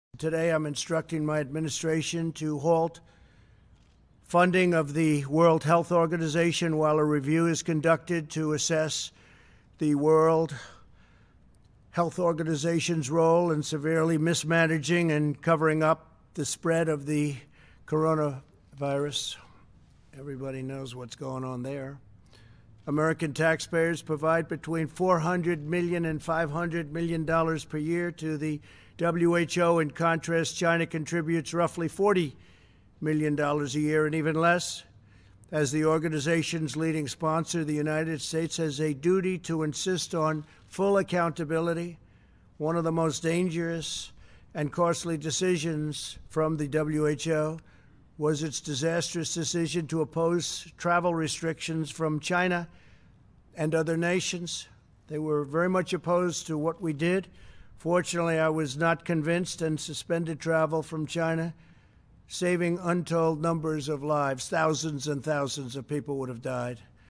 CV, Trump sospende i fondi all'Oms
Coronavirus, Trump sospende i fondi all'Oms

La decisione del presidente Usa che accusa l'Organizzazione di "gravi errori nella gestione della crisi"

 (Washington). "Oggi ho dato indicazioni alla mia amministrazione per fermare i finanziamenti destinati all'Organizzazione Mondiale della Sanità, mentre viene portata avanti una revisione volta ad esaminare il ruolo dell'Oms nella gestione negativa della crisi e nel tentativo di coprire la diffusione del coronavirus". Così il presidente degli Stati Uniti, Donald Trump, in una conferenza stampa alla Casa Bianca in cui ha accusato l'OMS di "gravi errori nella gestione della crisi" sanitaria e di "aver insabbiato la diffusione del virus". Il tycoon si è scagliato anche contro la Cina che sostiene l'Oms "solo" con 40 milioni di dollari all'anno, a fronte dei 400/500 milioni di dollari pagati dai contribuenti americani. Immediata la replica del segretario generale dell'Onu, Antonio Guterres, che ha invocato "all'unità e alla collaborazione da parte di tutta la comunità internazionale". (Laura Loguercio/alanews)